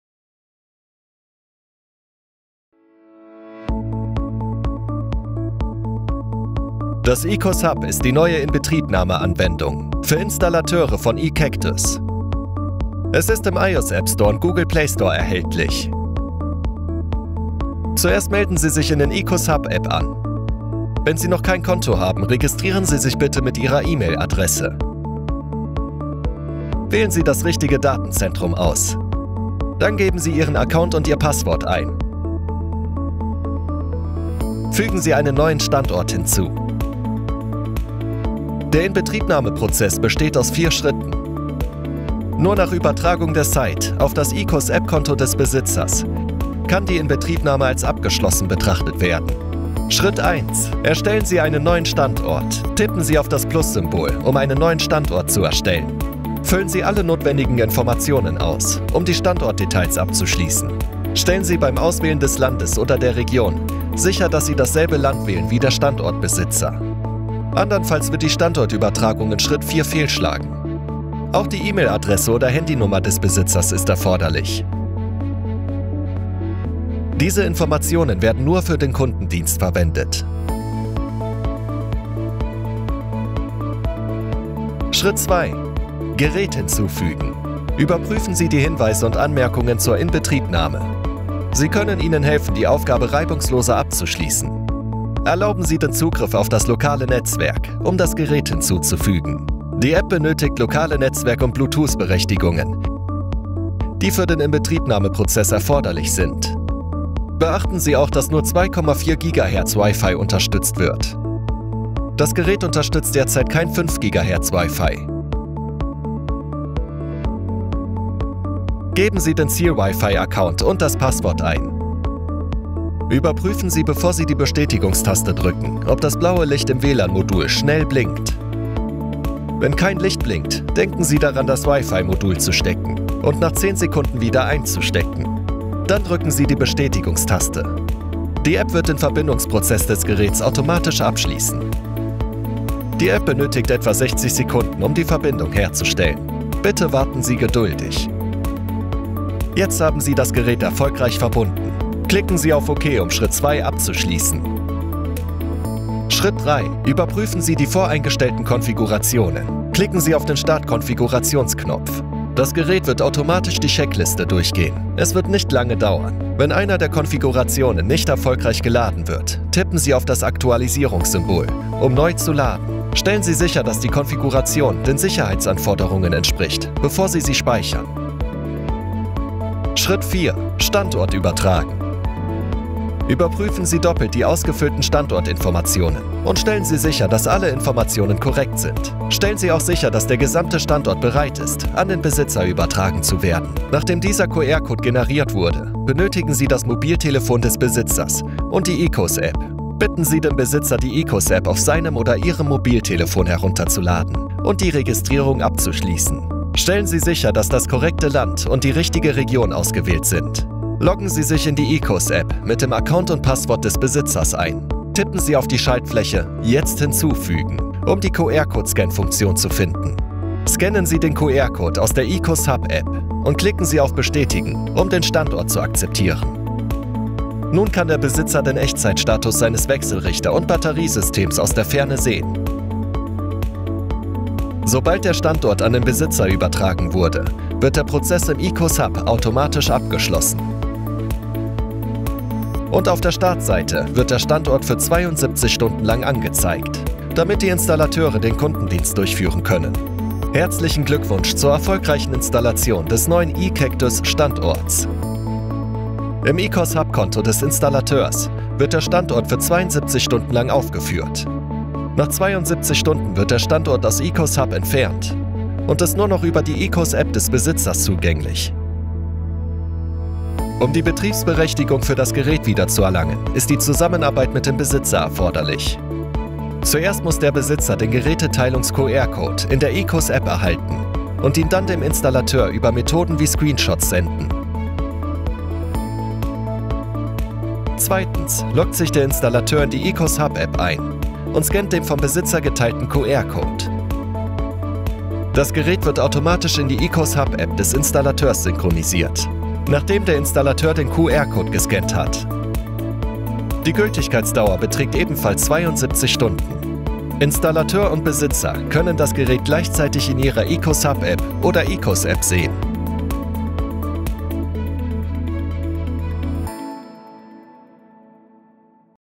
ECACTUS ECOS Hub – Installation & Konfiguration | Deutschland
ECOS Hub, ein Energiemanagementsystem, hilft Distributoren und Installateuren, ihr Energiespeichersystem aus der Ferne zu verwalten.